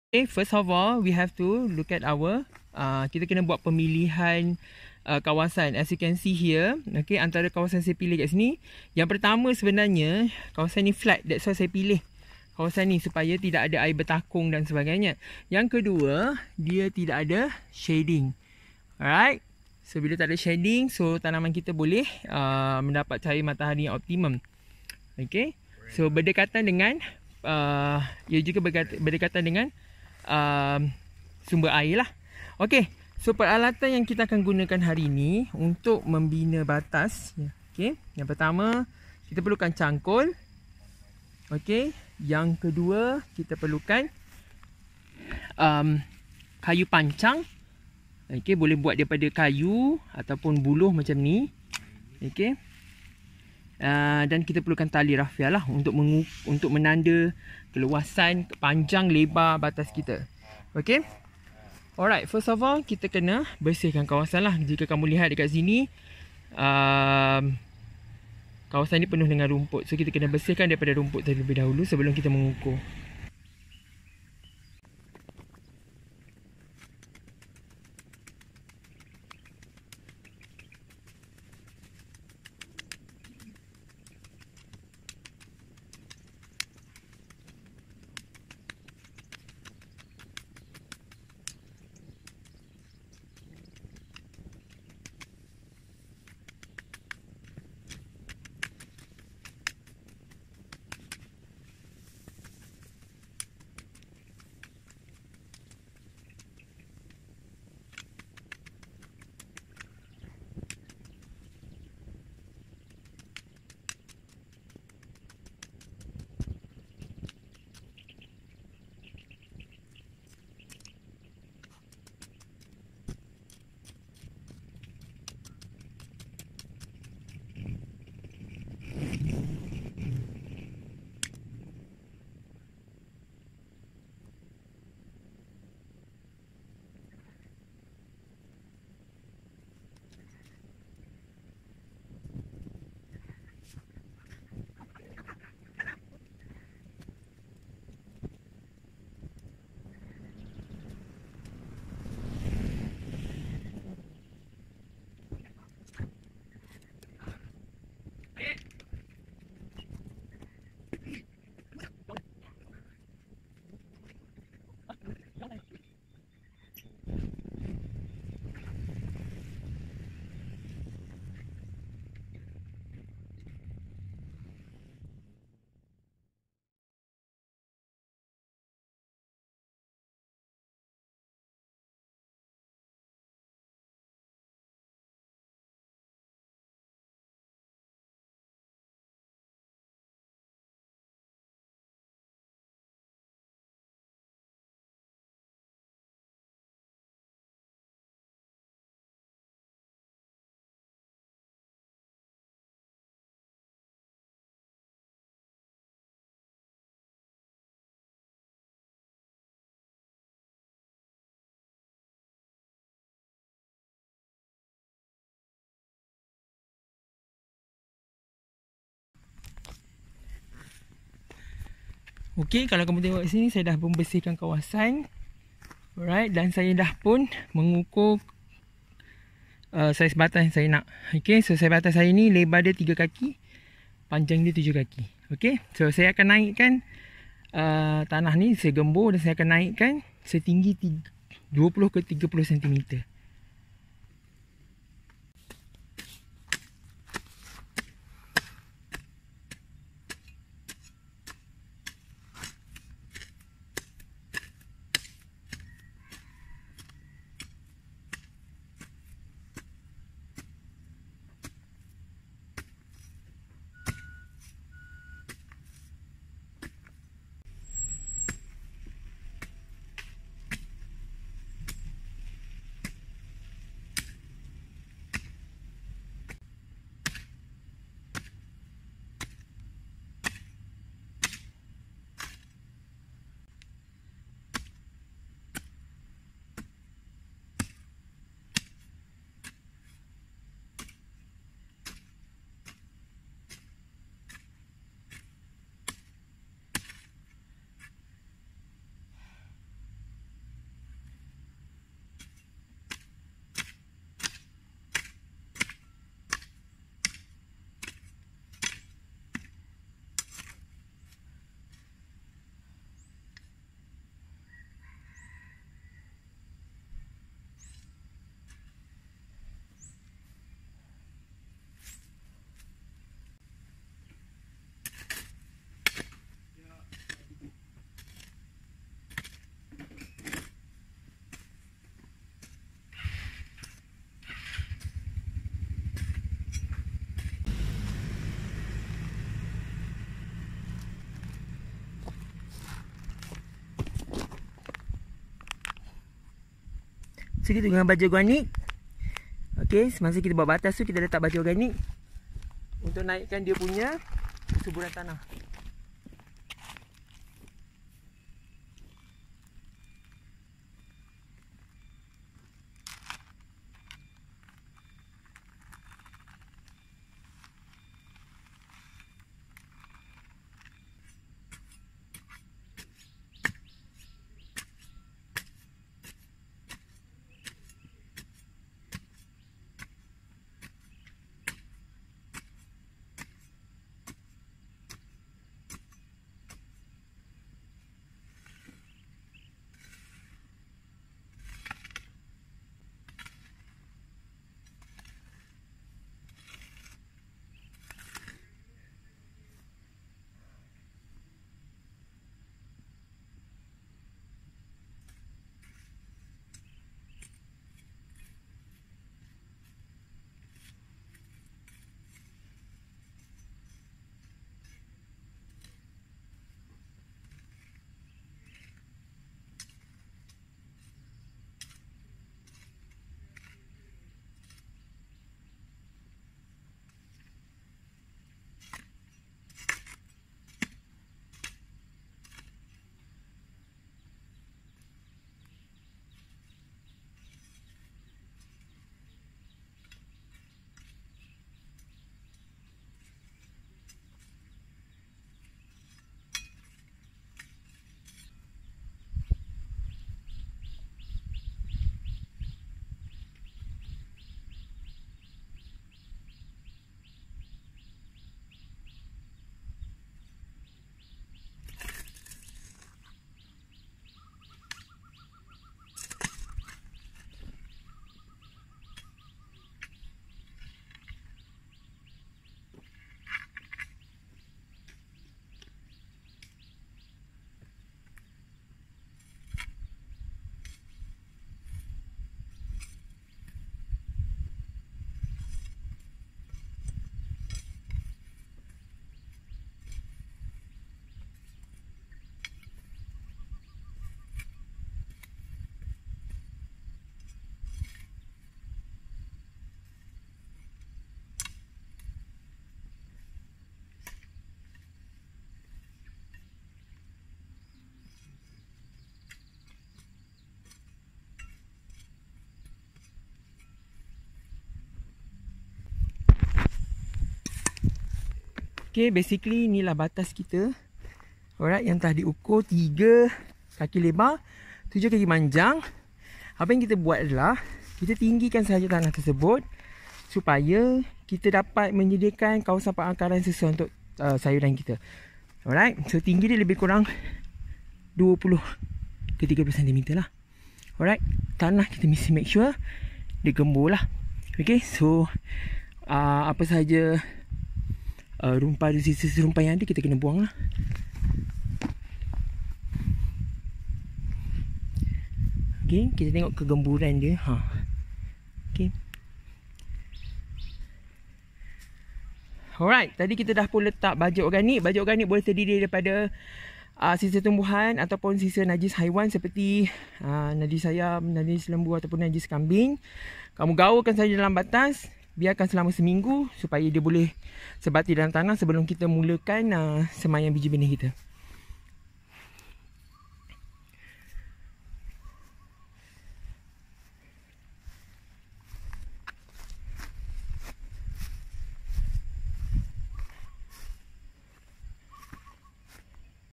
PLANTING BED PREPARATION
Simple tutorial on how to prepare  a conventional planting bed, tools you might need, and suitable size for planting a vegetables.